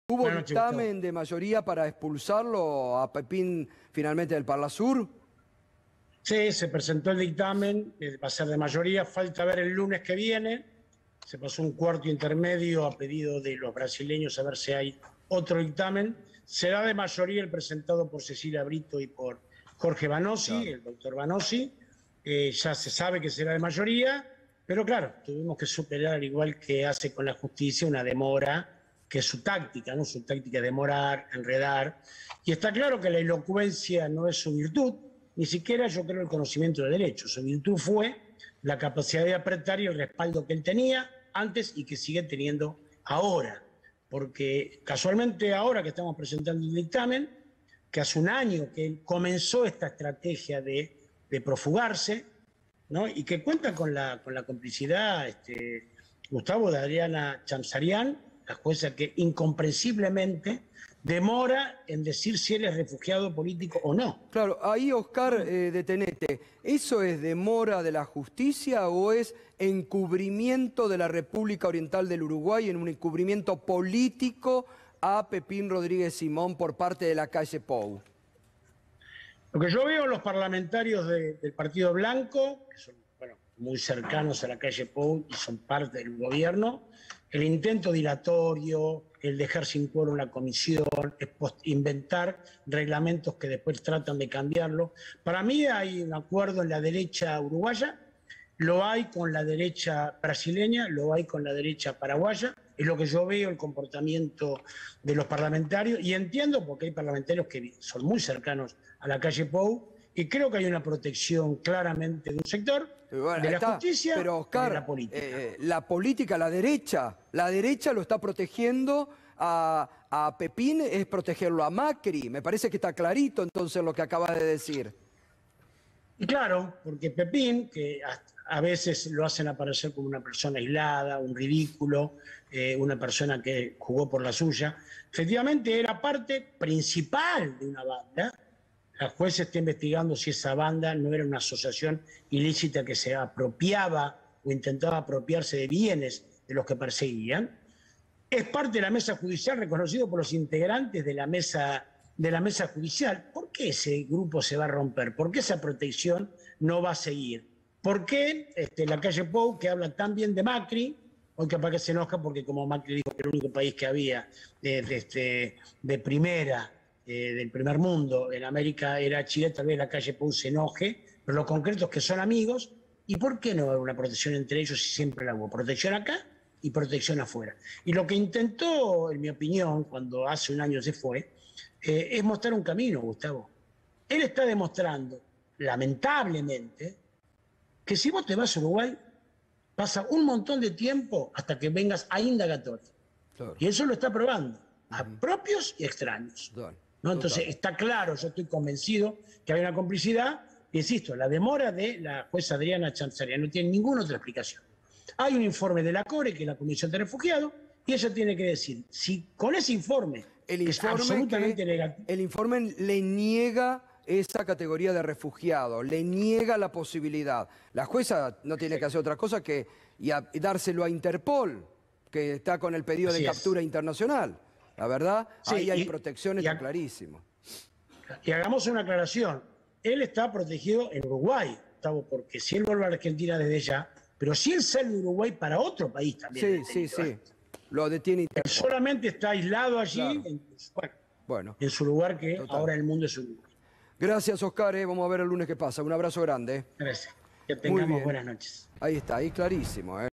C5N Aniversario de Pepin Prófugo
Cumplimos un año sin que el principal operador judicial macrista se presente a nuestra justicia.